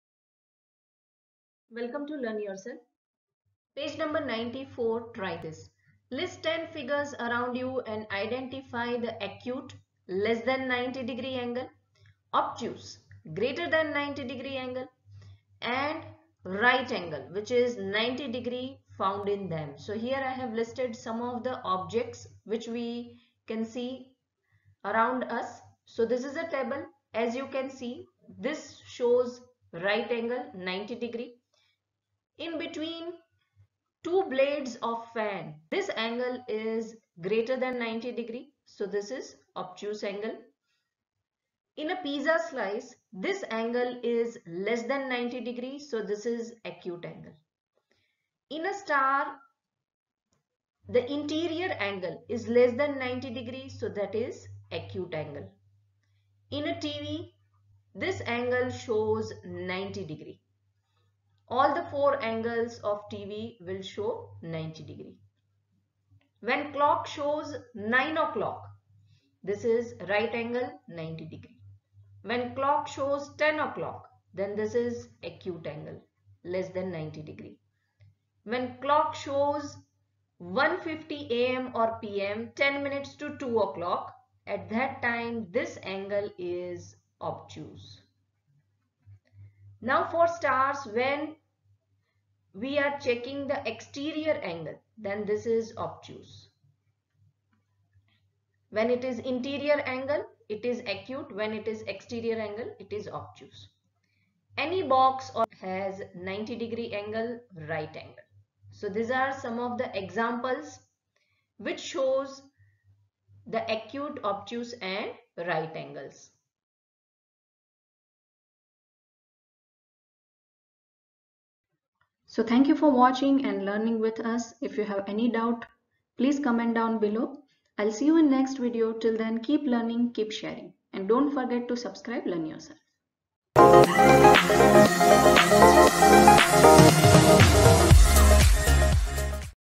Page no.94 Try These Lines and Angle Chapter-5|| CBSE STD-7 Math Ch-5|| Try These solutions
List ten figures around you and identify the acute, obtuse and right angles found in them.